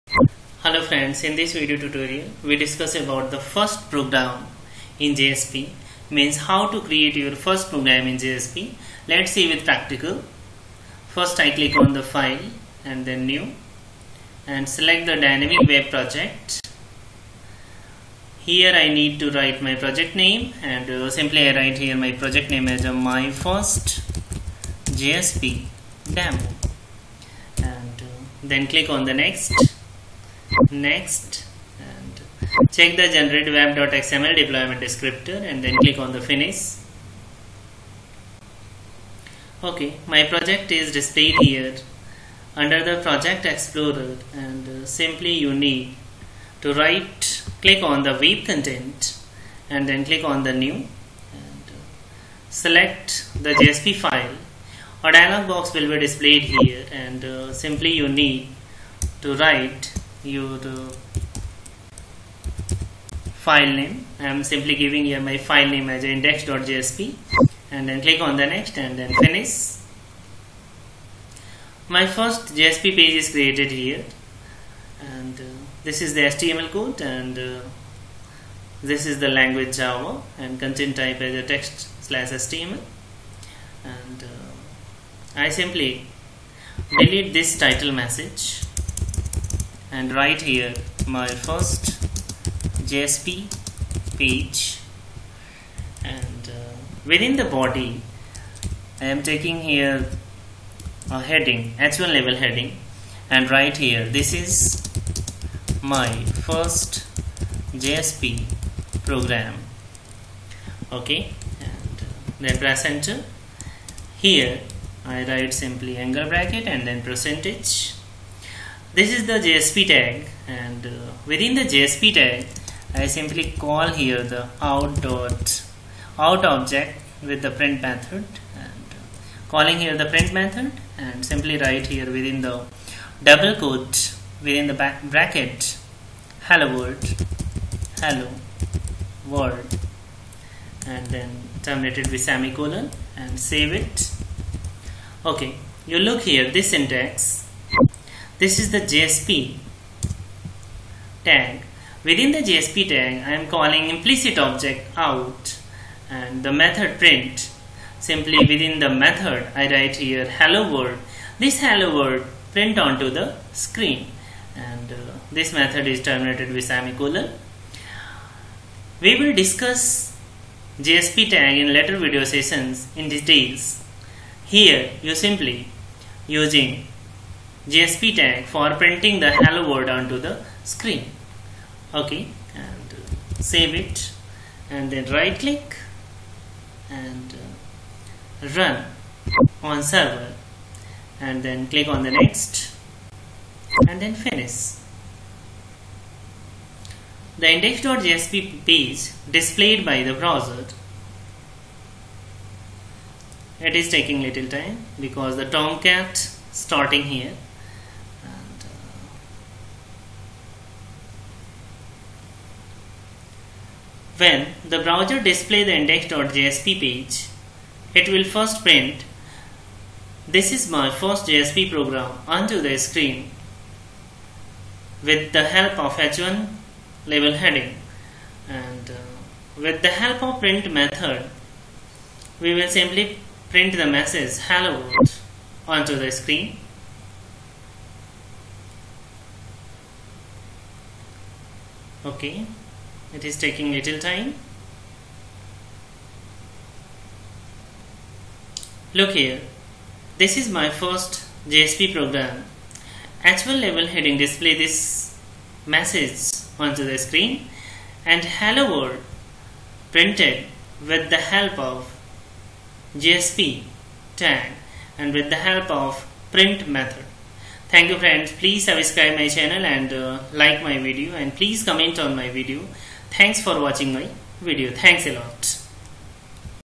first jsp program in eclipse part-3 How to Create JSP Page in eclipse | JSP Tutorials
In this video tutorials I demonstrate how to run your first jsp program using eclipse step by step